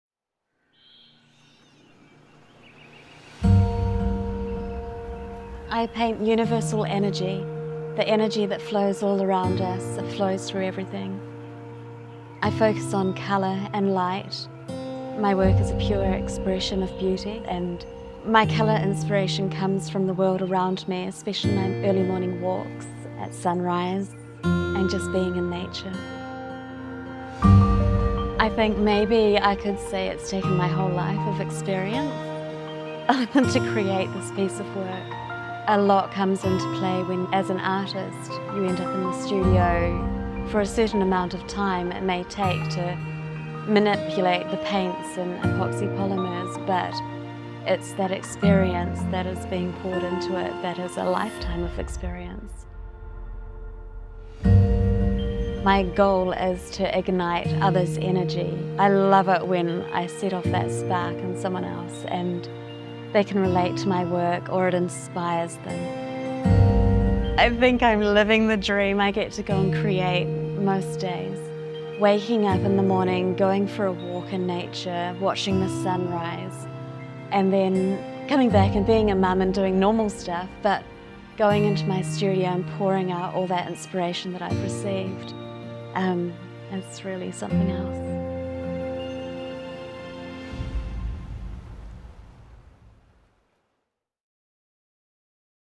NZ Artist Hollie O'Neill shares her painting inspiration from her Auckland studio.
NZ Artist Hollie O'Neill paints in her Auckland studio. She creates engaging and immersive large and mid-scale abstract color field paintings that celebrate the optical and emotional aspects of luminosity and color.  Painting universal energy, her paintings capture the emotional impact of nature's beauty.
and leading galleries in New Zealand including Ora Gallery in Wellington, Flagstaff Gallery in Auckland, and Zeayou Gallery in Taupo.